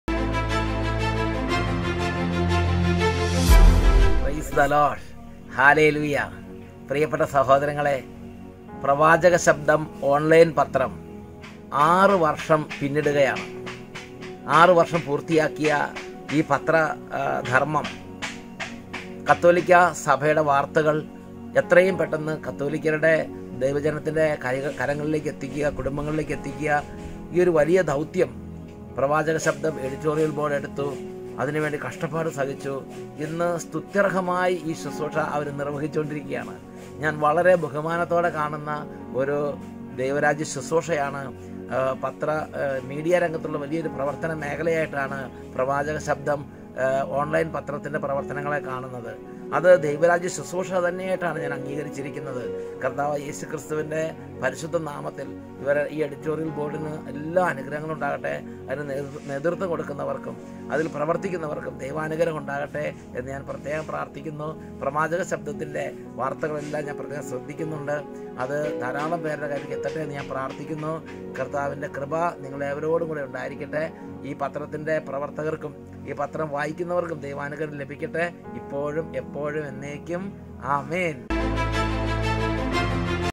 'പ്രവാചകശബ്ദം' ചെയ്യുന്നത് സ്തുത്യര്‍ഹമായ ദൈവരാജ്യ ശുശ്രൂഷ: ഫാ. സേവ്യര്‍ഖാന്‍ വട്ടായില്‍
''കത്തോലിക്ക വാര്‍ത്തകള്‍ വിശ്വാസികളിലേക്ക്, കുടുംബങ്ങളിലേക്ക് എത്തിക്കുക എന്ന ലക്ഷ്യത്തോടെ ആരംഭിച്ച 'പ്രവാചകശബ്ദം' സ്തുത്യര്‍ഹമായ സേവനം തുടരുകയാണ്. ഞാന്‍ വളരെ ബഹുമാനത്തോടെ കാണുന്ന ദൈവരാജ്യ ശുശ്രൂഷയാണ്, 'പ്രവാചകശബ്ദ'ത്തിന്റെ ശുശ്രൂഷകള്‍''.
കത്തോലിക്കാ മാധ്യമമായ 'പ്രവാചക ശബ്‌ദം' പ്രവർത്തനം ആരംഭിച്ചിട്ട് ആറു വർഷം പിന്നിട്ടിരിക്കുന്ന ഈ അവസരത്തിൽ ലോക പ്രശസ്ത വചനപ്രഘോഷകനും സെഹിയോന്‍ മിനിസ്ട്രീസ് സ്ഥാപക ഡയറക്ടറുമായ ഫാ. സേവ്യര്‍ഖാന്‍ വട്ടായില്‍ പങ്കുവെച്ച സന്ദേശം ടീമിന്റെ ശുശ്രൂഷകളെ പറഞ്ഞ കാര്യങ്ങള്‍ ശ്രവിക്കാം.